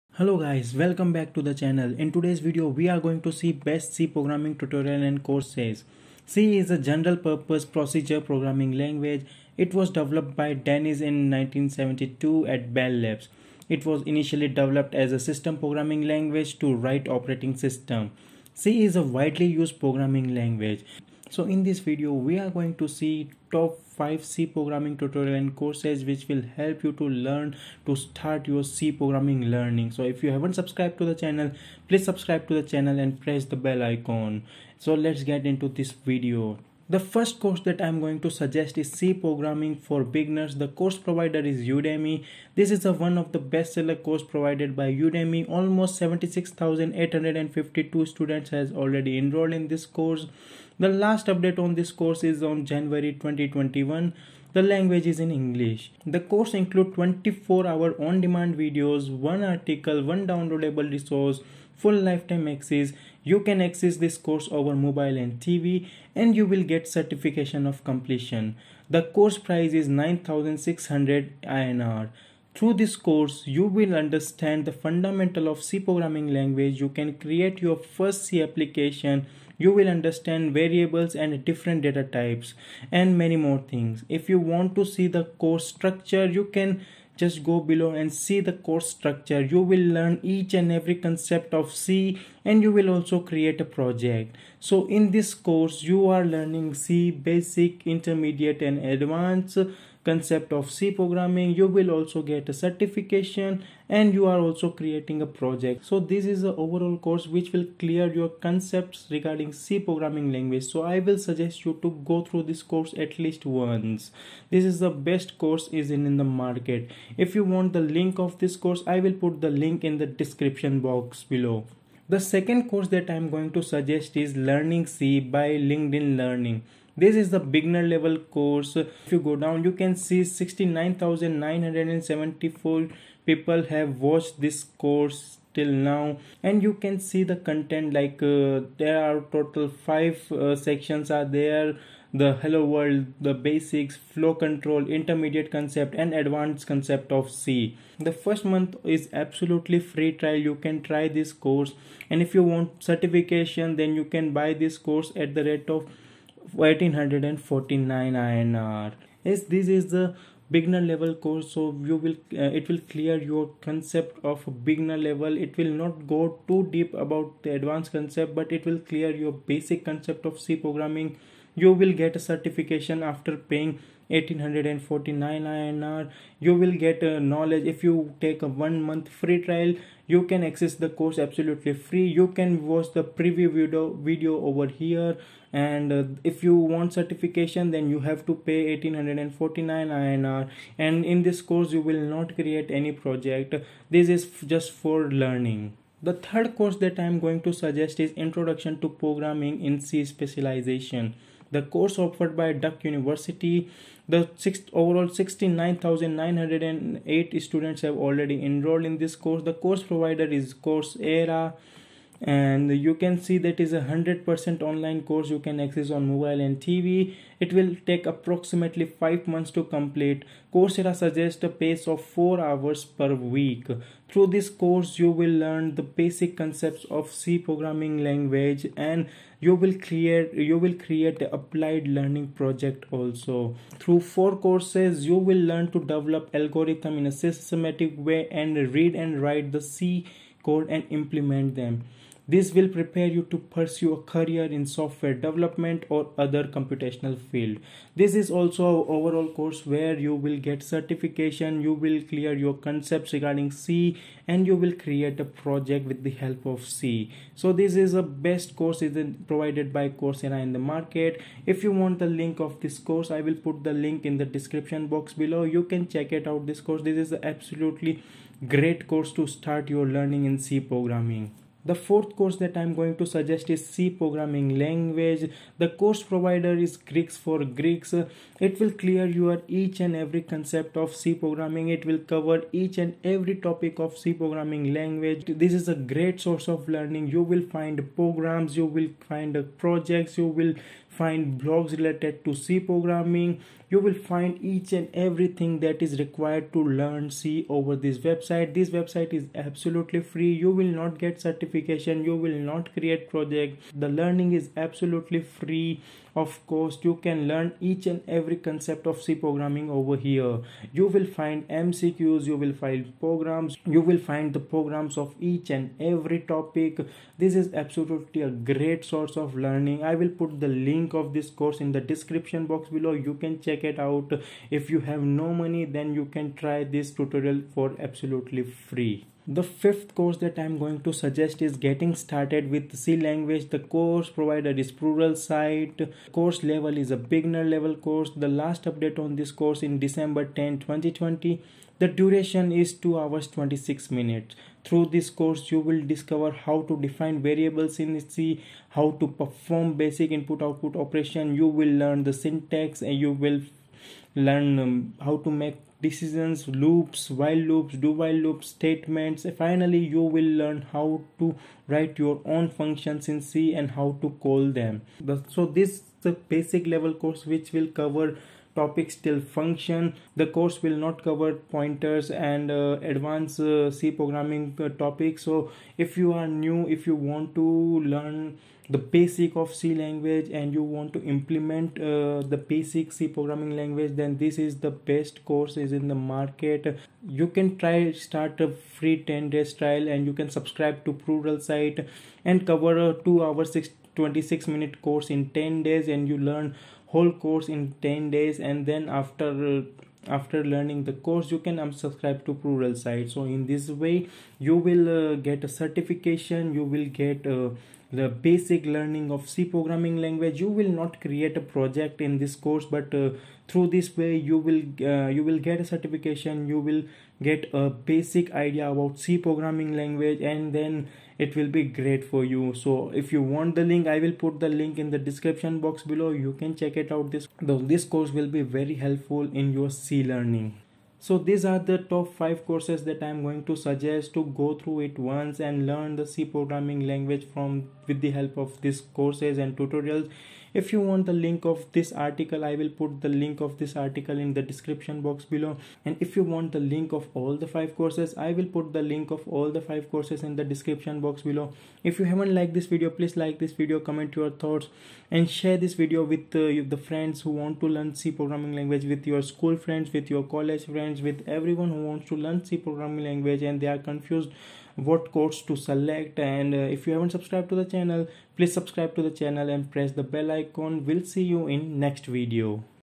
5 Best C Programming Language Courses & Tutorials - Letsfindcourse
Learn C Programming - Check out these 5 best C programming language courses and tutorials recommended by experts.

1. C Programming For Beginners - Master the C Language | Udemy
2. Learning C Online Class | LinkedIn Learning
3. Introduction to Programming in C | Coursera
4. C Programming Language | GeeksforGeeks
5. Getting Started with the C Language | Pluralsight